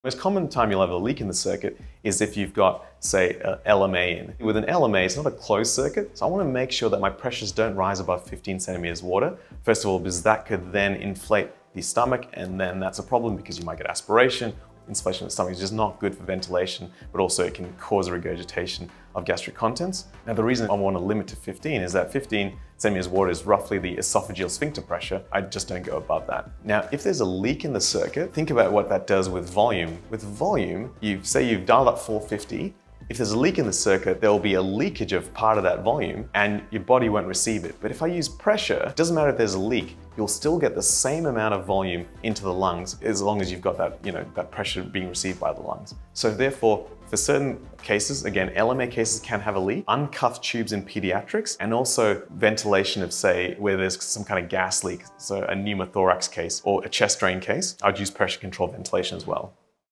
Use Pressure Control if Theres a Leak
For the full video, please check out "Simplifying the Complexities of the Anaesthesia Ventilator" at this